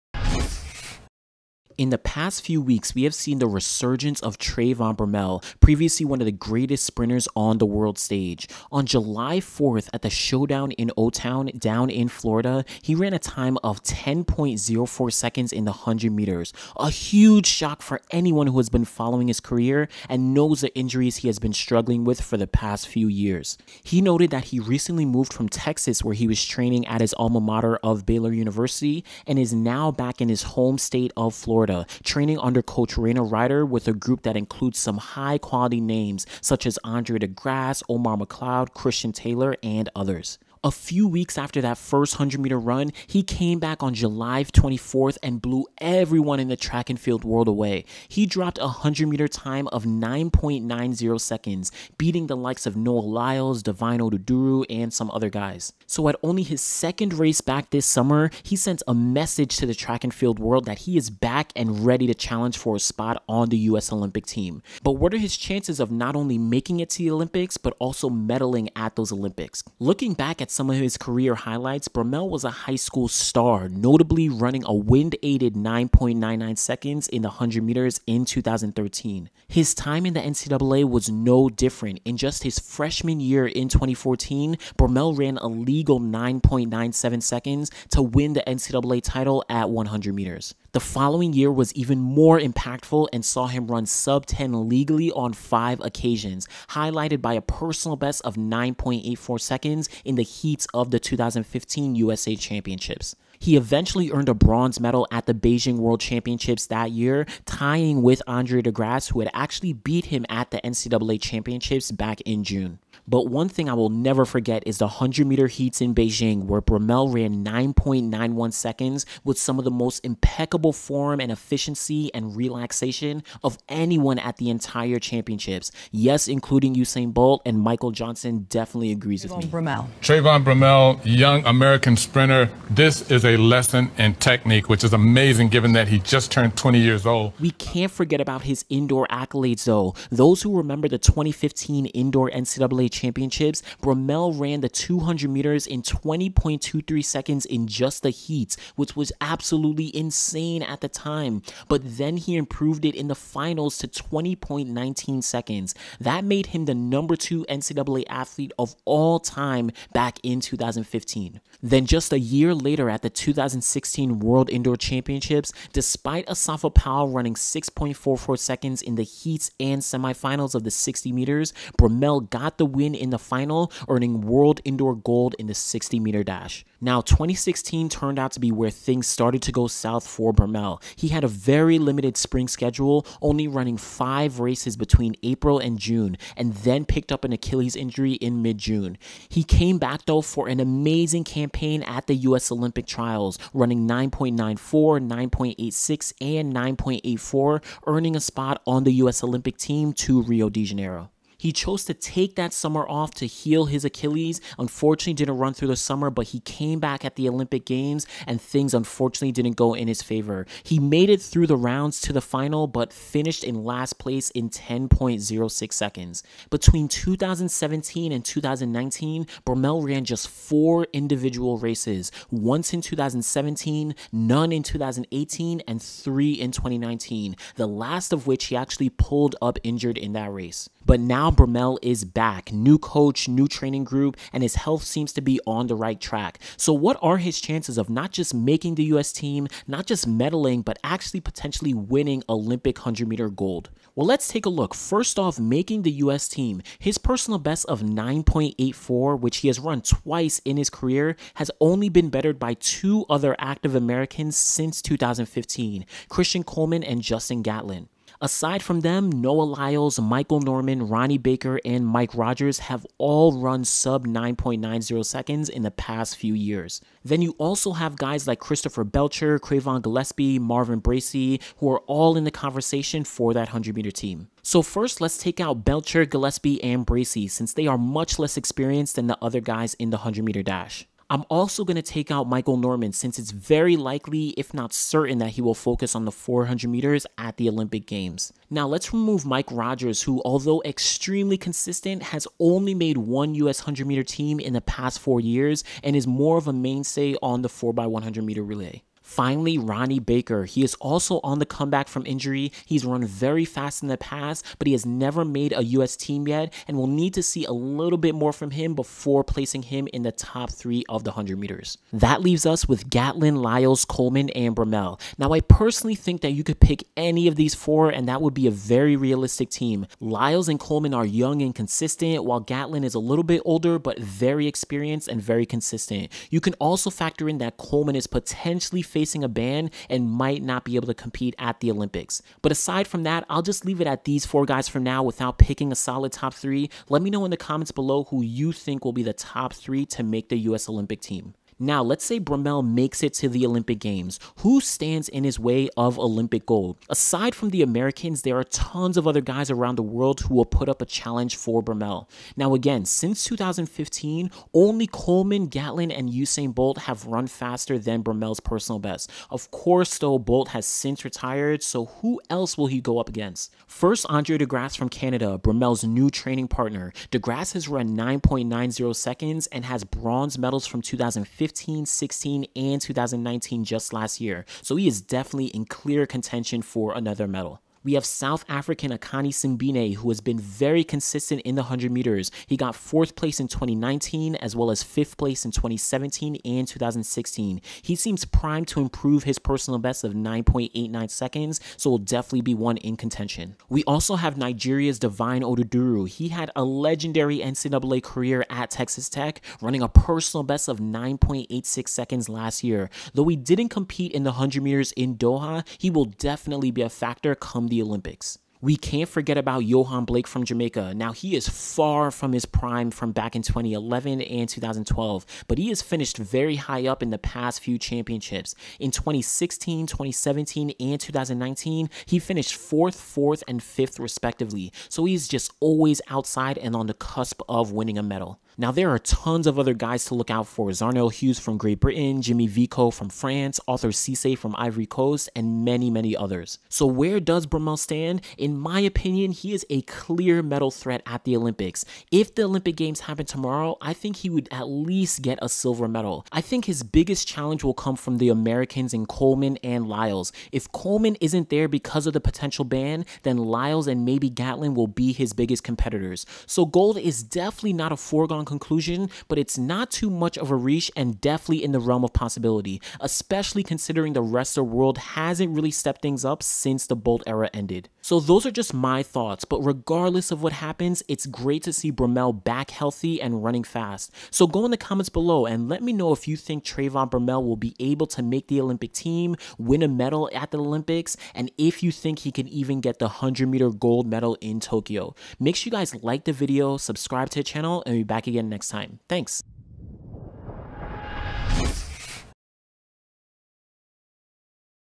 Can Trayvon Bromell Win a Gold Medal in the Tokyo 2021 Olympic Games?
Who is the fastest  runner in Tokyo 2021 Olympic